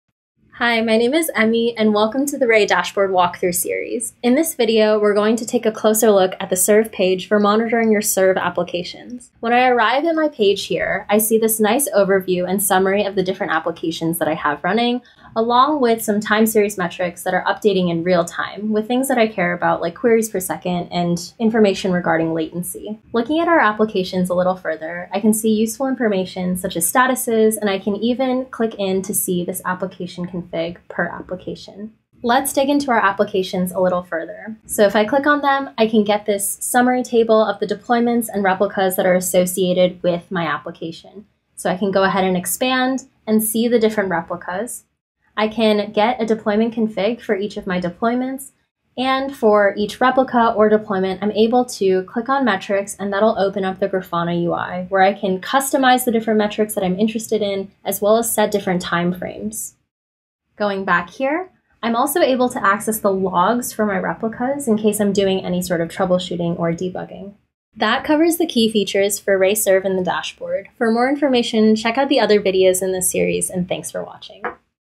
Ray Dashboard Series: Part Three | Serve
In this tutorial, Emmy will take you on a short tour of the Serve view in Ray Dashboard which allows you to gain insight into the three layers of Ray Serve: applications, deployments, and replicas.

Version: The Dashboard demonstrated here reflects Ray 2.4 and you can find the most up to date information in our docs!